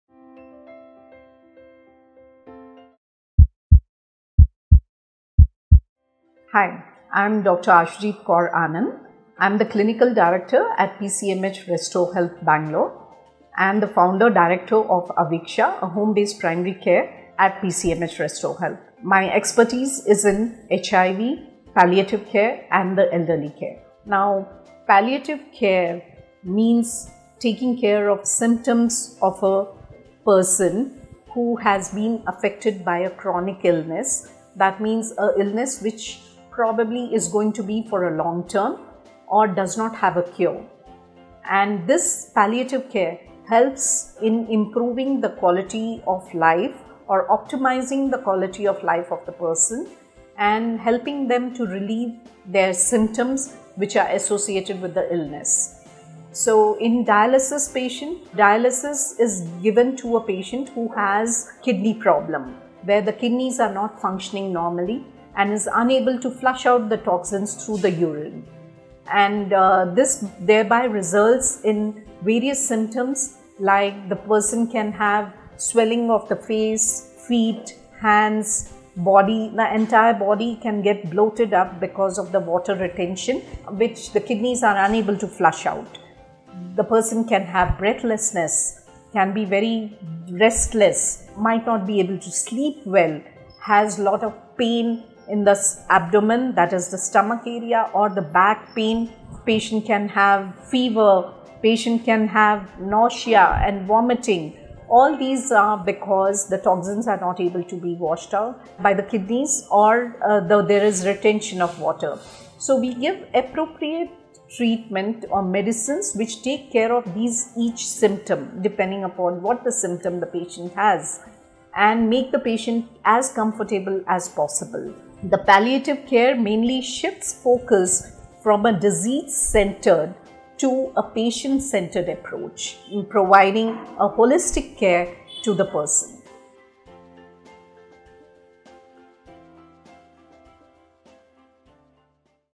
5 TIPS To Care | Palliative Care for Dialysis Patients - Dr. Ashoojit Kaur Anand | Doctors' Circle
Dr. Ashoojit Kaur Anand | Appointment booking number: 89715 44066
General Physician & Community Medicine Specialist | PCMH Restore Health, Bangalore
Palliative Care helps in improving  the  quality of life  or  optimizing the  quality of  life  of the person and helps to relieve their symptoms. Dialysis is  given to a patient with kidney problem and is unable to  flush out  the toxins and thereby  results in various symptoms .